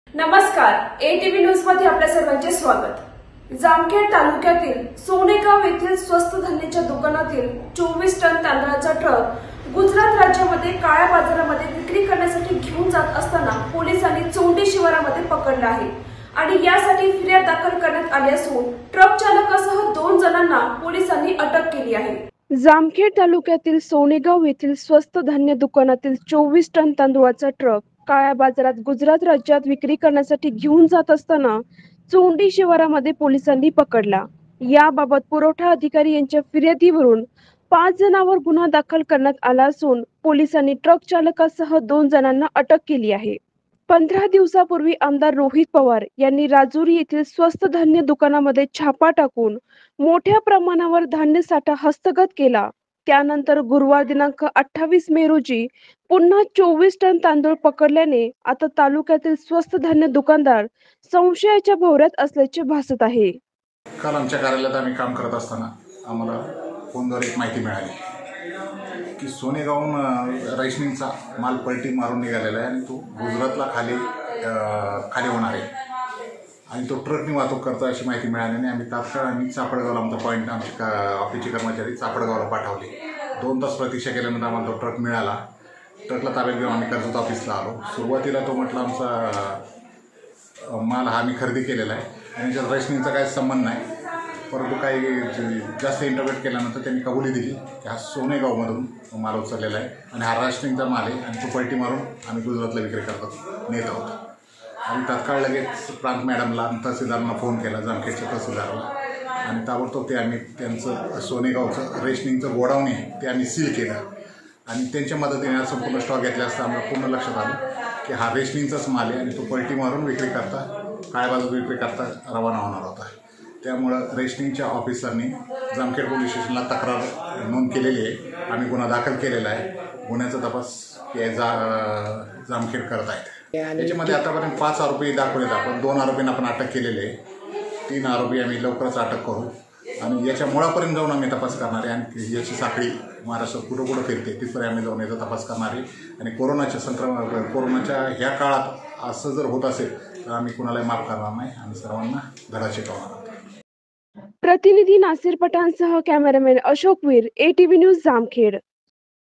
ATv News काळ्या बाजारात रेशनिंगचा २४ टन तांदूळ विक्री करण्यासाठी जात असताना जामखेड पोलिसांनी पकडला
Latest Ahmednagar City  NEWS update
जामखेड - तालुक्यातील सोनेगाव येथील स्वस्त धान्य दुकानातील २४ टन तांदूळाची ट्रक काळ्या बाजारात गुजरात राज्यात विक्री करण्यासाठी जात असताना चोंडी शिवारात पोलीसांनी पकडला याबाबत पुरवठा अधिकारी यांच्या फिर्यादीवरून पाच जणांवर गुन्हा दाखल करण्यात आला असून पोलिसांनी ट्रक चालकासह दोन जणांना अटक केले आहे. पंधरा दिवसांपूर्वी आ. रोहीत पवार यांनी राजुरी येथील स्वस्त धान्य दुकानात छापा टाकून मोठ्या प्रमाणावर धान्य साठा हस्तगत केला होता त्यानंतर गुरुवार दि. २८ रोजी पुन्हा २४ टन तांदूळ पकडल्याने तालुक्यातील स्वस्त धान्य दुकानदार संशयाच्या भव-यात असल्याचे भासत आहे.